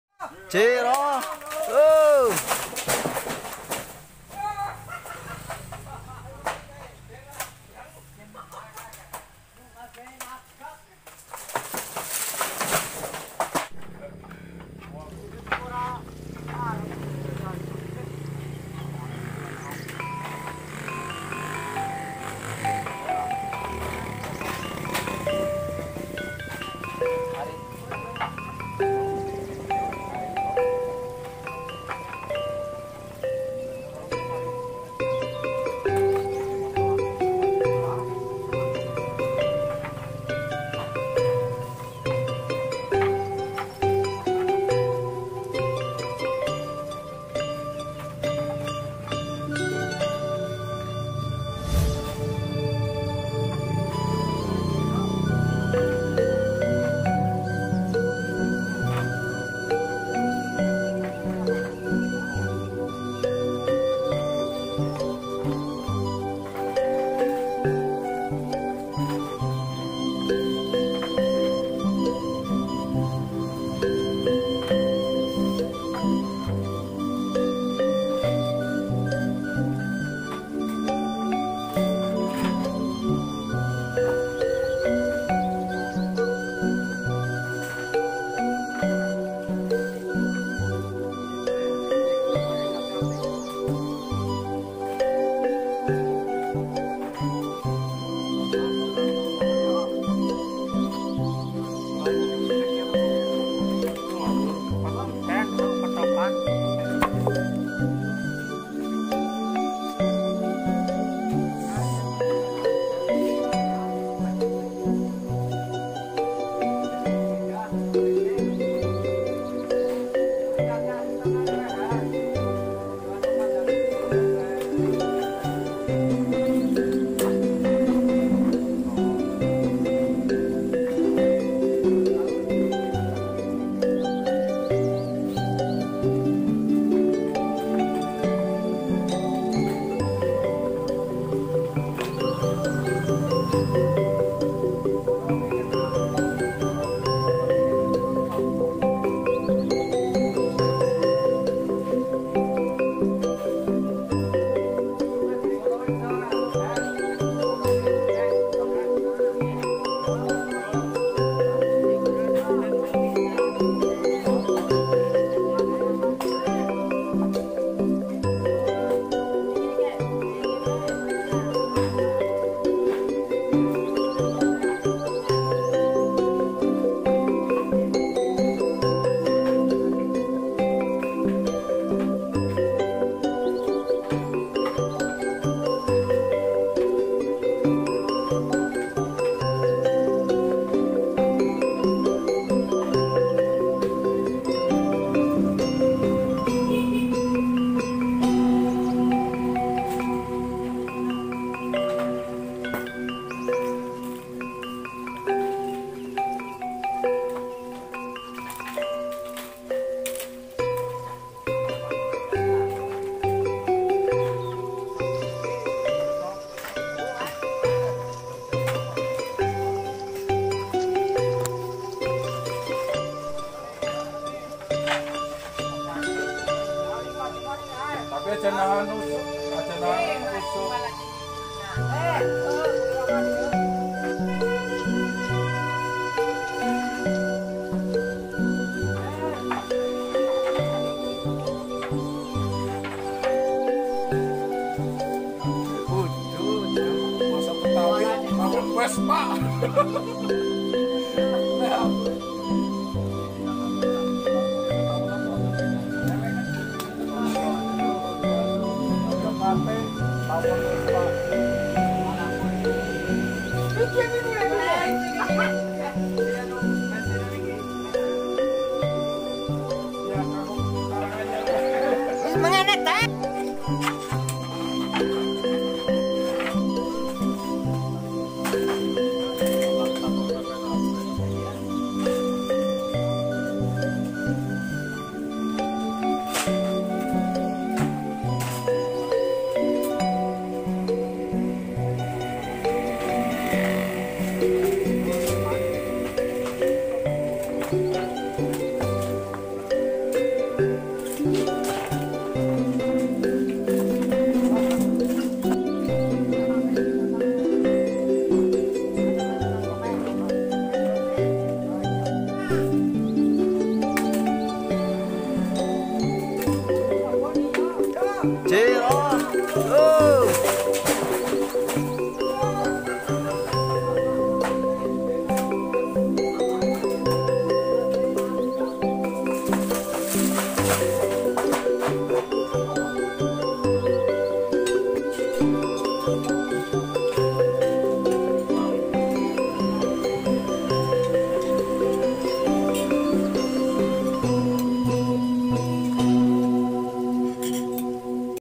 Gedubrakkk.... || Gotong Royong Bongkar Rumah Warga
kondisi rumah sudah mau ambruk, banyak kayu yang lapuk, akhirnya warga bergotong royong untuk menurunkan genteng dan merobohkan rumah untuk dibangun kembali